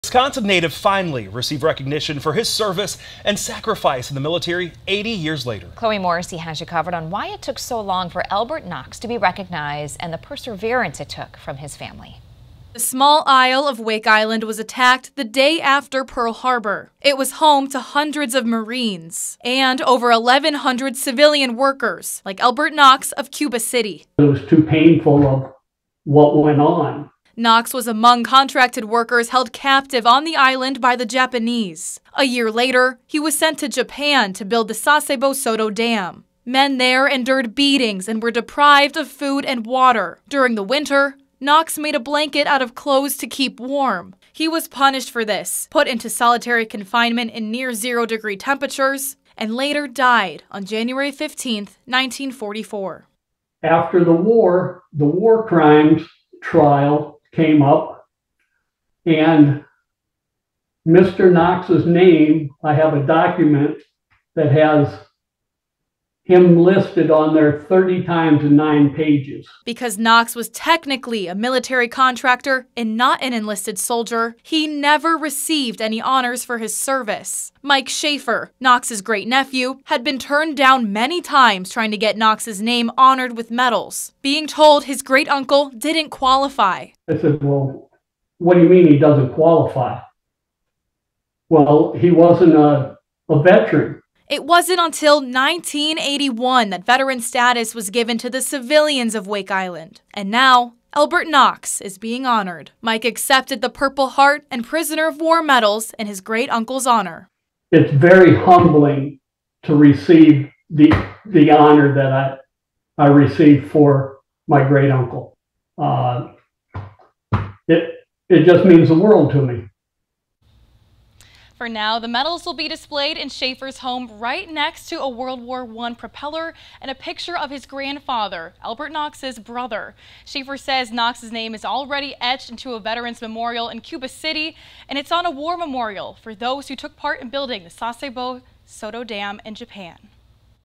'It just means the world to me': Great-nephew of late Cuba City WWII veteran accepts Purple Heart,
A WWII veteran from Cuba City, WI, has finally been honored for his service and sacrifice - 80 years later.

Family members of Elbert H. Knox received a Purple Heart and Prisoner of War medals on his behalf. His great-nephew, Mike Shaffer, received the awards at the Naval Air Station Joint Reserve Base Fort Worth.